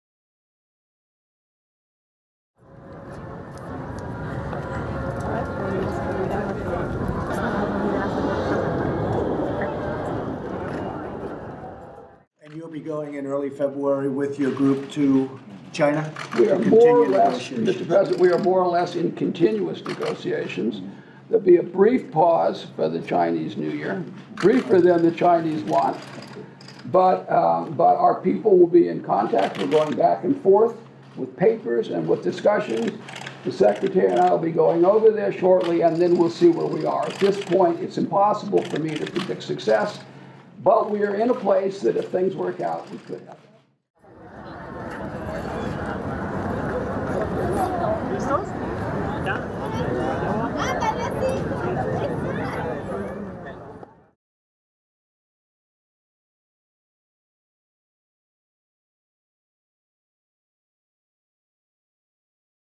US trade negotiators to visit China in February--Trump
"You will be going early February with your group to China?" asks Donald Trump to negotiators after they spent a second day of talks with Chinese counterparts.